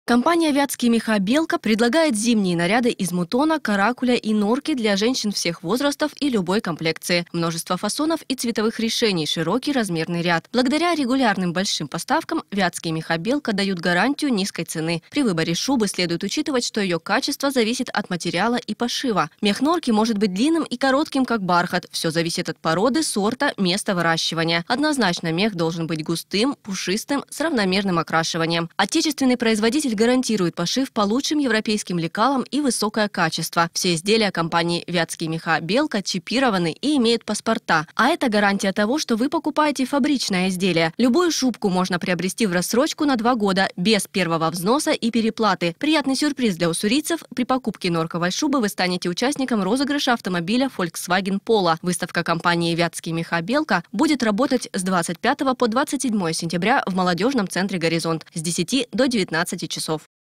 Акцию для своих покупателей устроила компания "Вятские меха. Белка"
Автомобиль в подарок. Акцию для своих покупателей устроила компания "Вятские меха. Белка". При покупке норковой шубы уссурийцы могут рассчитывать на ценный приз. Принять участие в розыгрыше можно с 25 по 27 сентября.